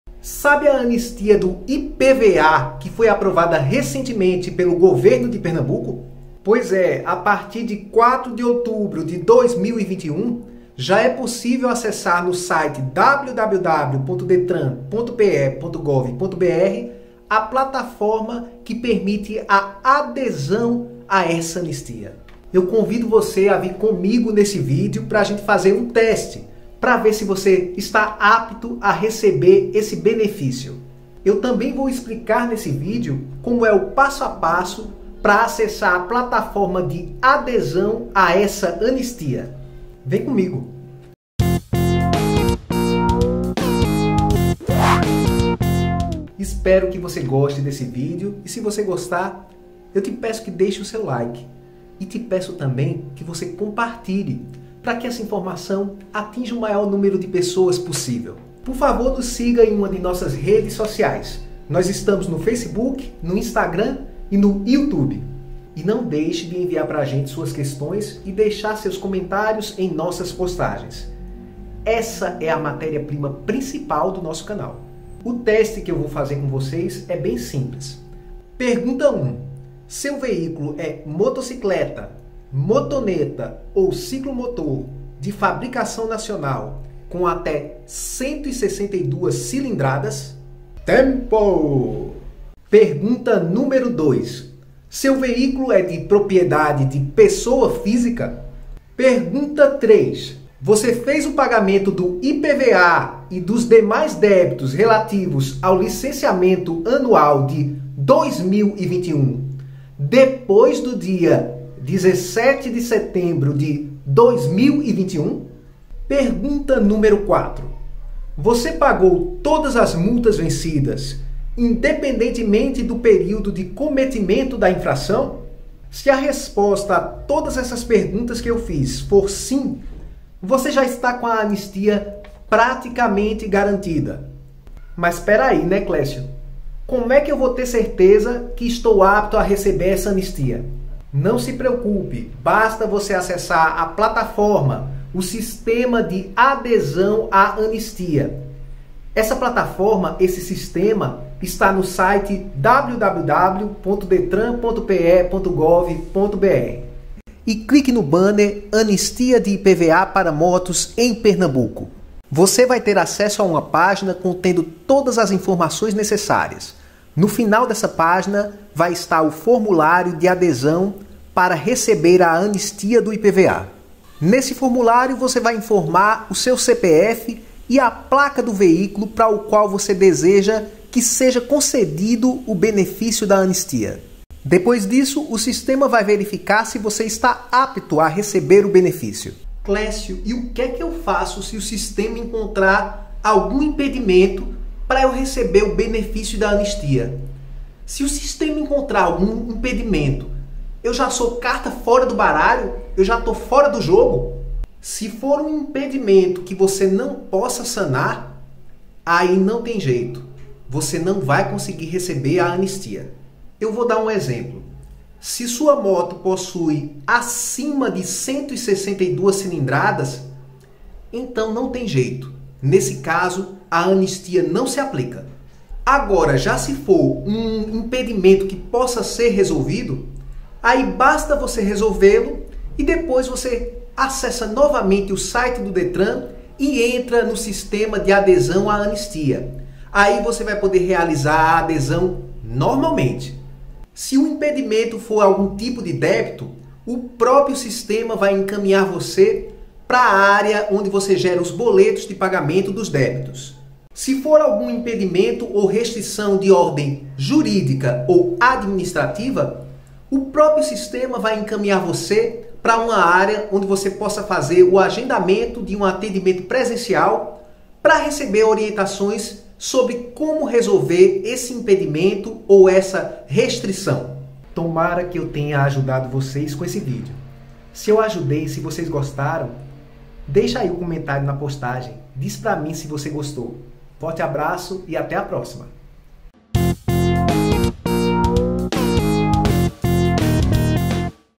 Anistia de IPVA para motos já está valendo em Pernambuco
A Anistia de IPVA para motos já está valendo em Pernambuco. Saiba como  fazer a adesão para receber o benefício com o Analista de Trânsito.

Ajude esse projeto a se manter.